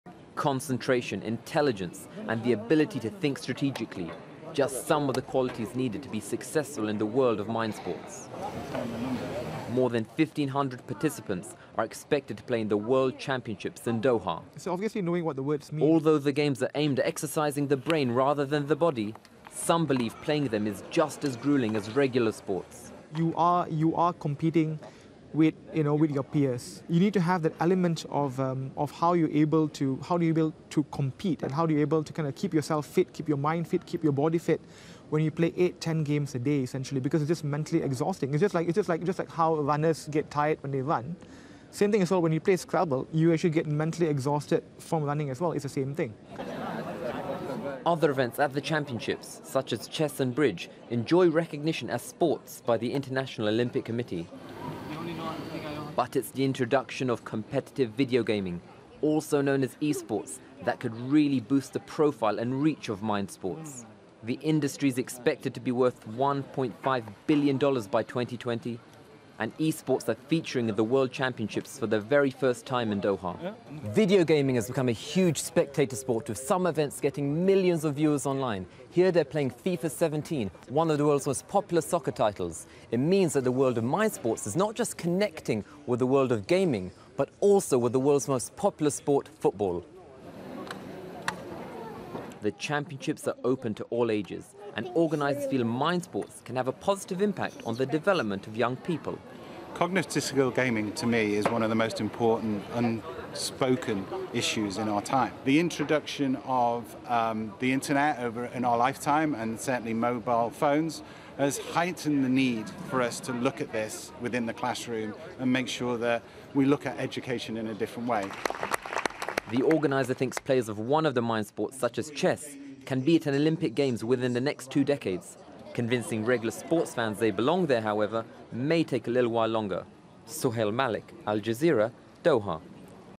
Mindsports 4th World Championships kicks off in Doha
The Mindsports World Championships began in Doha on Saturday. This is an event where physical activity isn't key to participation - with scrabble and chess the headline events.

Al Jazeera's Sohail Malik reports from Doha.